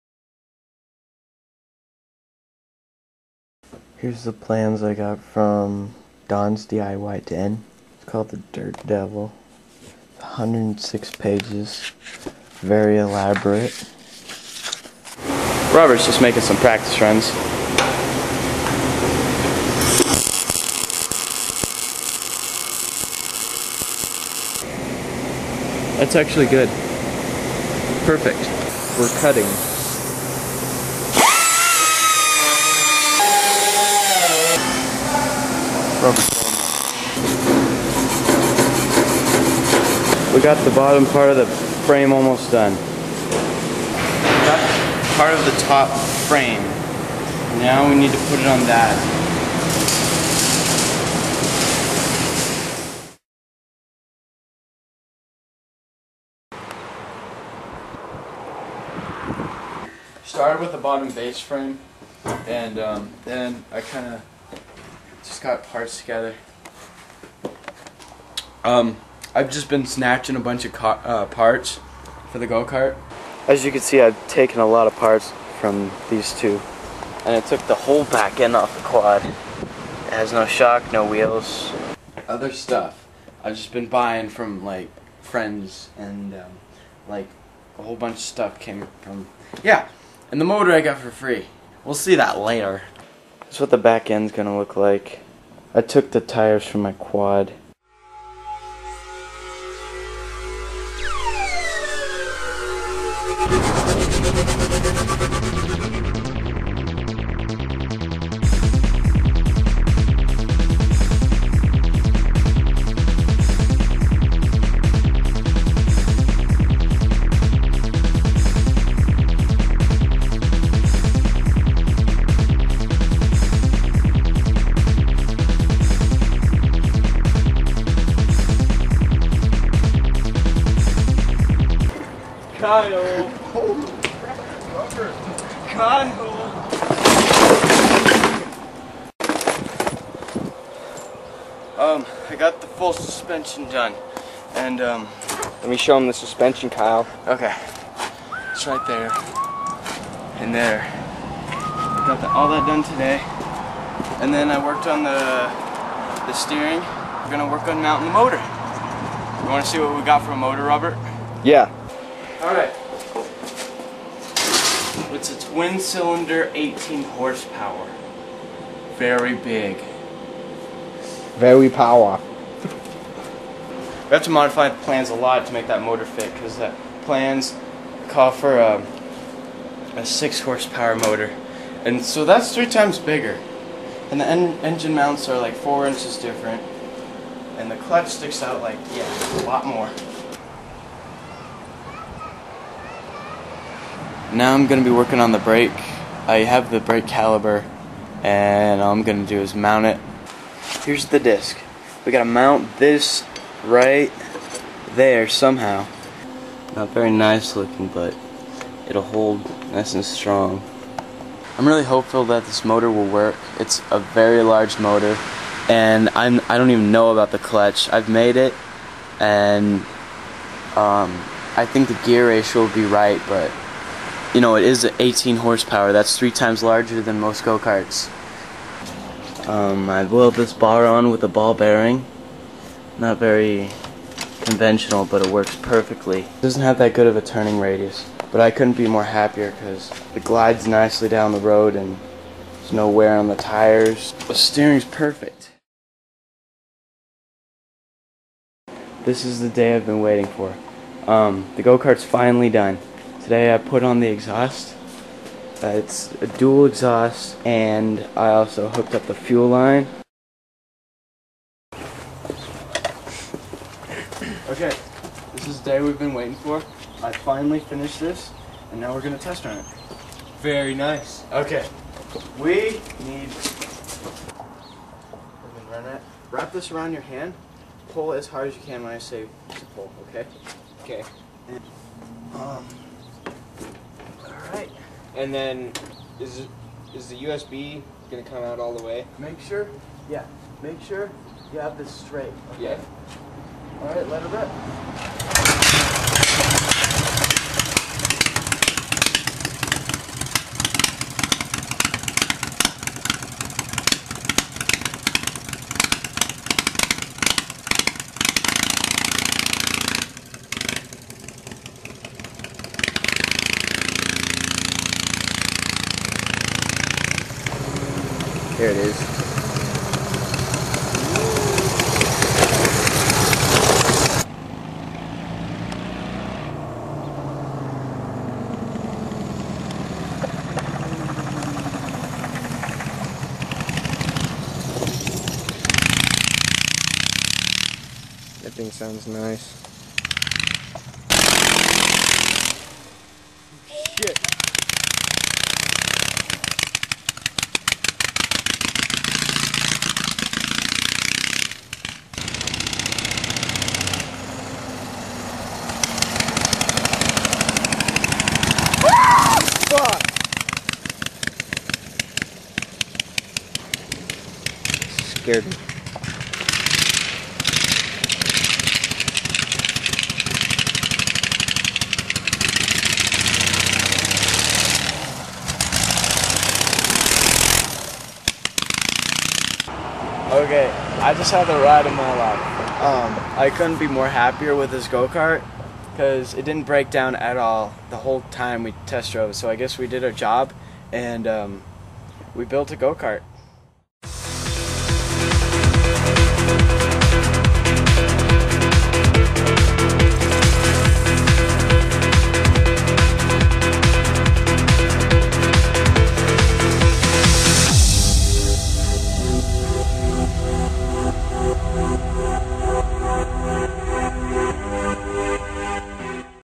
Kylemonkey Builds a Go-Kart (extended)
This project was done for a graduating grade at my high school. the grade on it was a 98% and i got a 95 on my speech (which was 12 minutes long.) this was the video i showed during my presentation.

this project took a total of 2 to 3 months of work, and i started with ordering a set of go-kart plance from Don's DIY Den (diyden.net.) The go-kart was origionally designed for a 5 horsepower motor, but i put in a monster 18 horsepower twin cylinder motor. it wasnt the BEST idea because the ngine is on the side and it is kind of off weight. other than that and the crappy breaks, there are no real problems with it.

The frame used mostly .095" one inch square tubing, and for the braces and brackets, i used 1/4" sheet metal.

All of the parts in which i did not build i obtained from northern tool (northerntool.com.) some parts like the front wheels and steering wheel, i got from friends or yard sales.

This thing started up the first pull and yes, i know the mixure is off, but once we got this thing working, it topped out at about 50 MPH, andhad real nice handling. not that great of a turning radious, but good handling.

the clutch was actually a belt clutch and i welded on a sprocket, and it worked perfectly. it is a single speed which is similar to second gear. im DYING for a two pully centrifugal clutch system so i can get all my horsepowers out.

i have done substantial work since this video. the front shocks DO look bent. theyre not. single A-arm suspentions do move around like that. i admit it does look dumb, SO i put some real beefy shocks up front, and it did give it a four or five inch lift. and now it doesnt look bent anymore. it went from lookling like
this:
to this
Rob and i welded on it using some TIG a lot of MIG, but mostly flux chore welding. i used PAC to cut some pieces and i used a hydraulic sheer to cut and bend a lot of the bracings, mounts, and brackets.